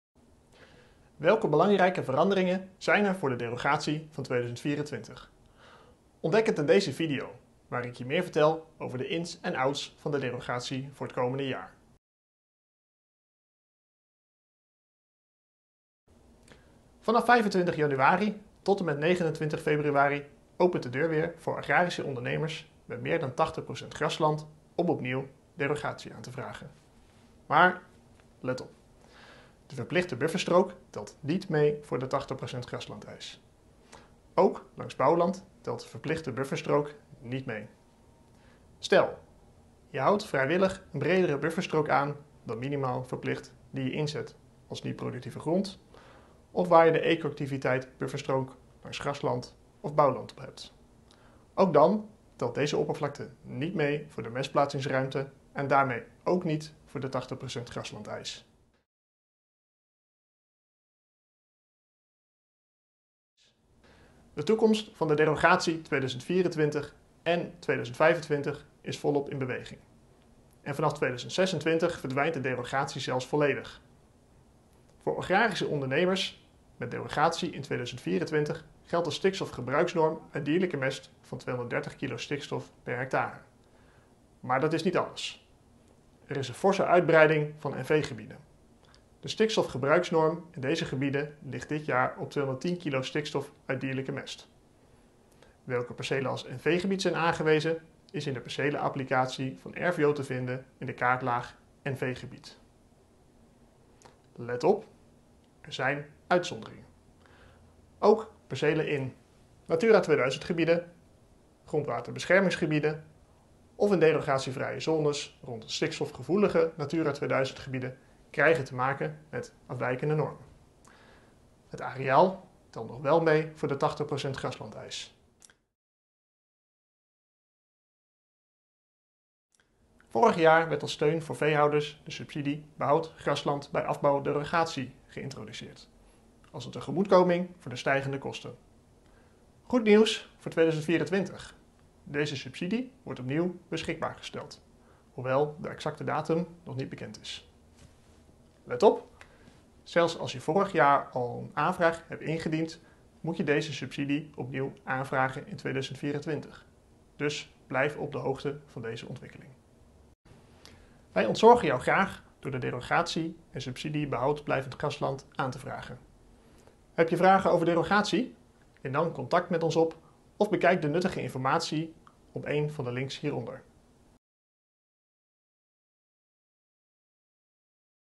Derogatie 2024
De derogatie wordt stap voor stap afgebouwd en eindigt definitief in 2026. De derogatienorm is in 2024 lager dan in 2023: 210 kg stikstof voor de NV-gebieden en 230 voor de rest van Nederland. Je kunt de vergunning voor 2024 aanvragen van 1 tot en met 29 februari 2024. Bedrijfskundig adviseur Wouter Vieveen heeft een video opgenomen, waarin hij je meeneemt in de belangrijkste veranderingen van de derogatie voor 2024. Kijk je mee?